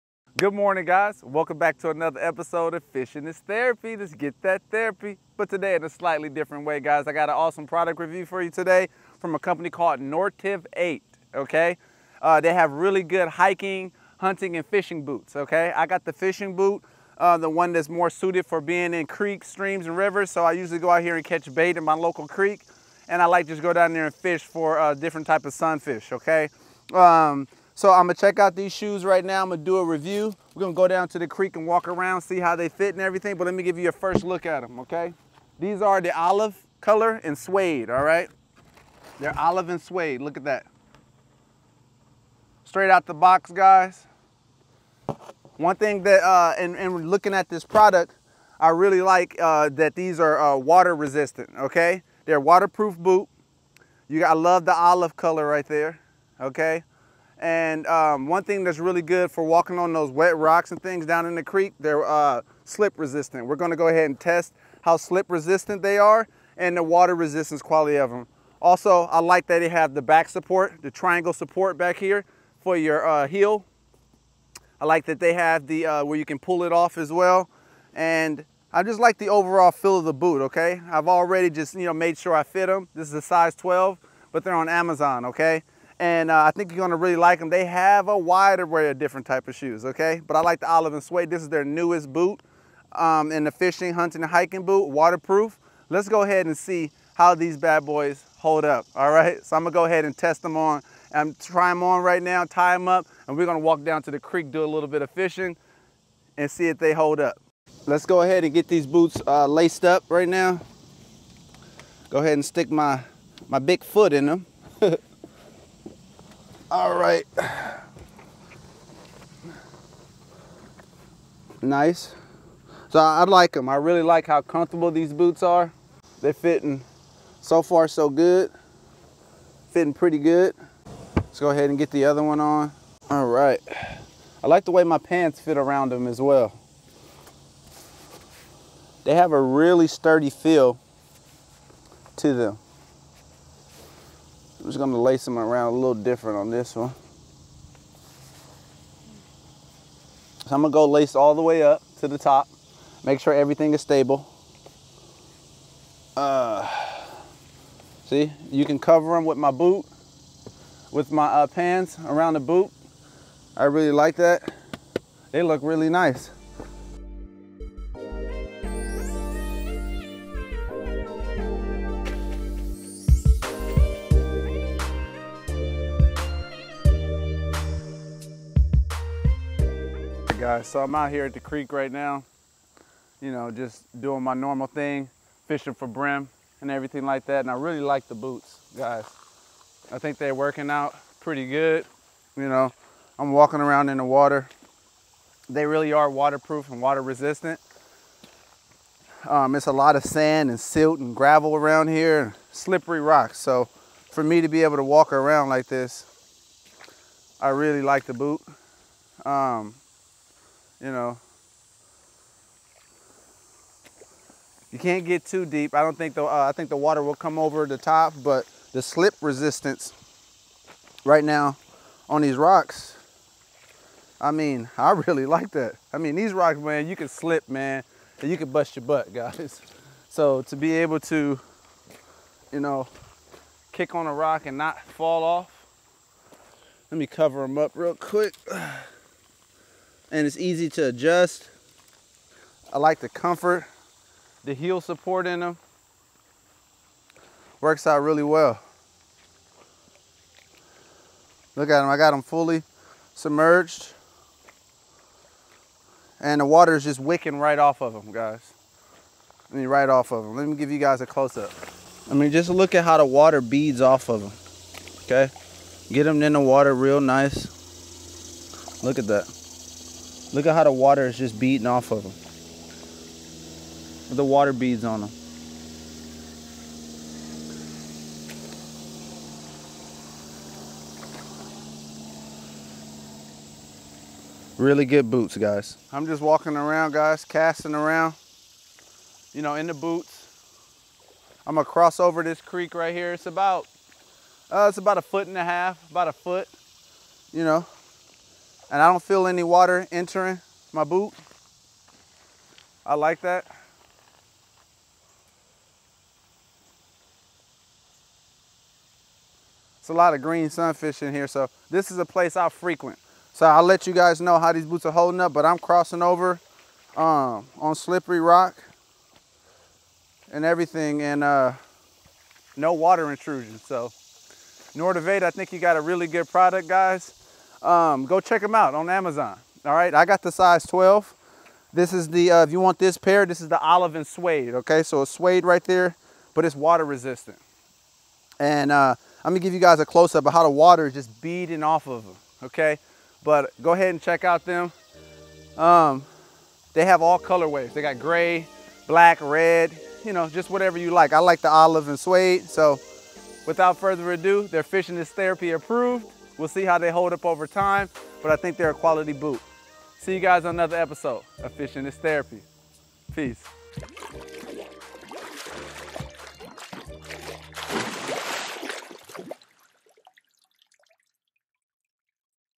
NORTIV 8 Men's Ankle High Water-Resistant Boots- A Versatile Fishing and Hiking Boot
On Todays episode of Fishing Is Therapy we review the Nortiv 8 Fishing and Hiking Anke High Boots.

*If yow ant a versatile hiking and fishing boot at a great price, then these are for you.

Pick Up Yours Today!
NORTIV 8 Men's Ankle High Water-resistant Hiking Boots Outdoor Lightweight Shoes Trekking Trails Armadillo